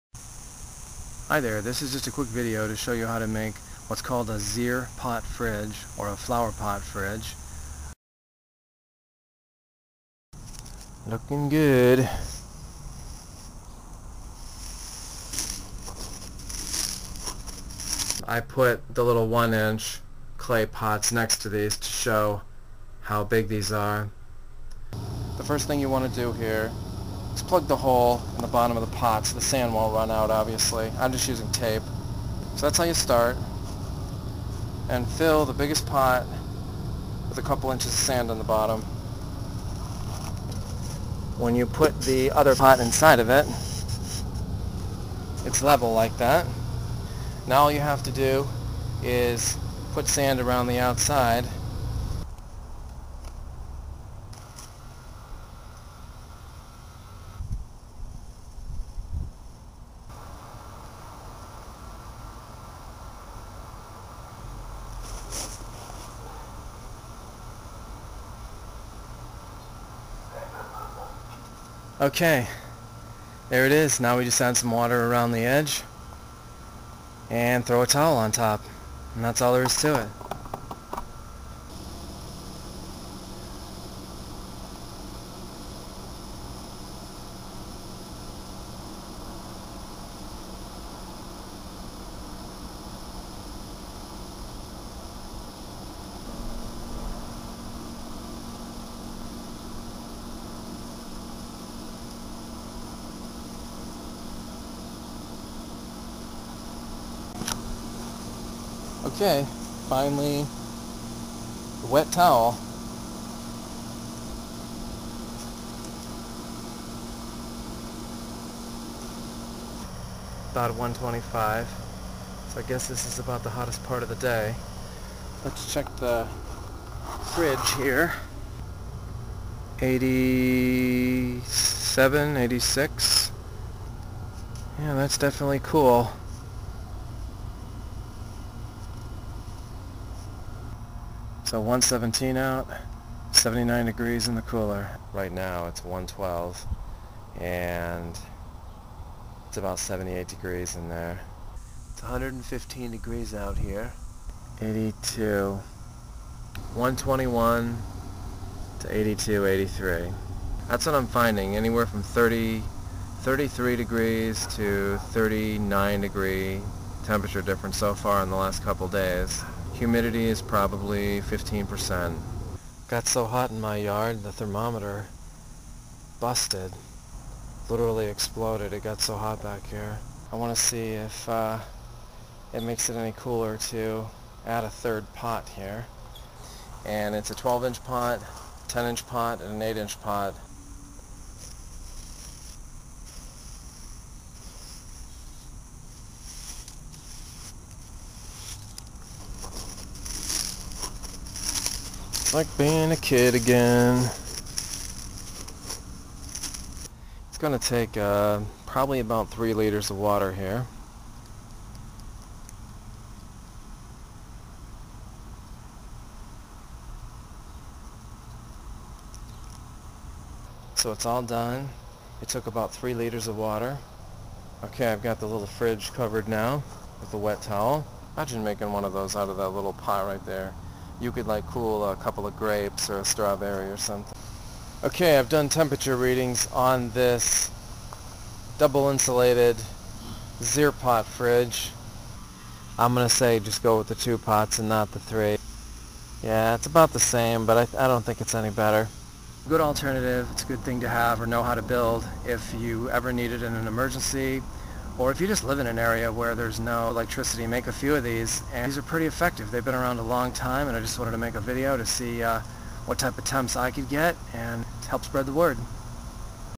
Homemade Pot-in-Pot Refrigerator "Off Grid Fridge" cools air up to 40F (evaporative cooler/chiller)
pot-in-pot refrigerators (zeer pot coolers) will cool the air up to 40 degrees.  no power needed.  uses only 2 or 3 clay pots, sand, water and a towel.  will cool the inner chamber up to 40 degrees (relative to the outside air).  uses no electricity.  will keep food fresh longer. note:  i tested it when it was 120 degrees out.  if you use it when the temperature is 80F-90F it will get down to 40F to 50F.